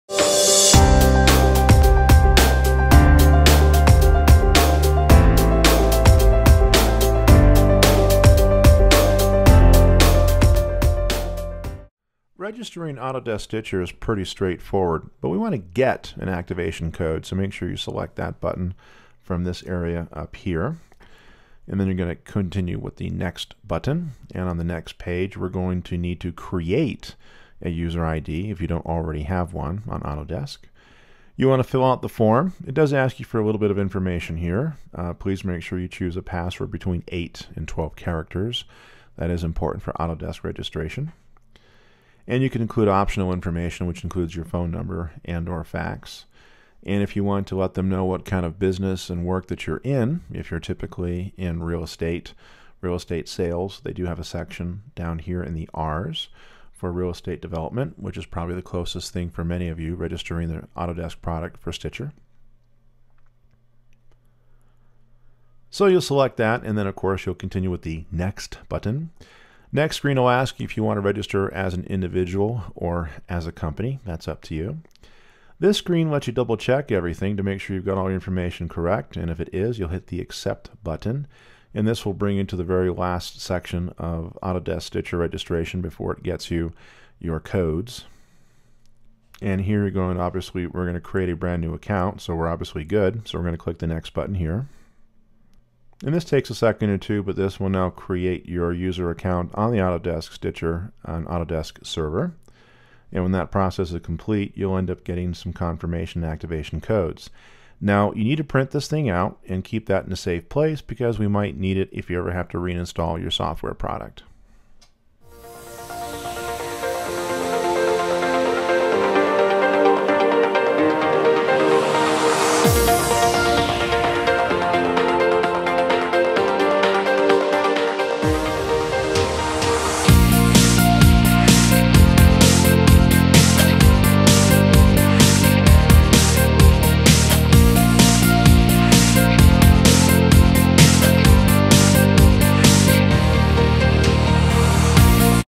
How to Register Autodesk Stitcher Unlimited
Watch this short video on how to register Autodesk Stitcher Unlimited 2009 for the Mac or PC.  Once you have your Autodesk Stitcher Code:  Serial Number: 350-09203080    and     Product Key: 551A1